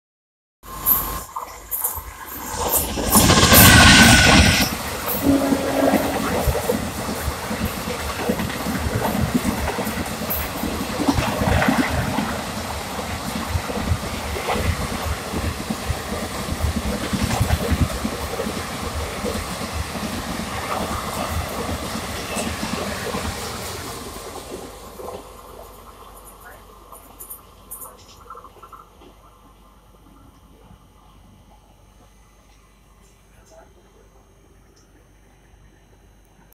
New Delhi - KSR Bangalore Karnataka Express aggressively speeds through ChaneraWAP-4/12628
My tripod
My Camera
My travel accessories
My Backpack

Like the capture ?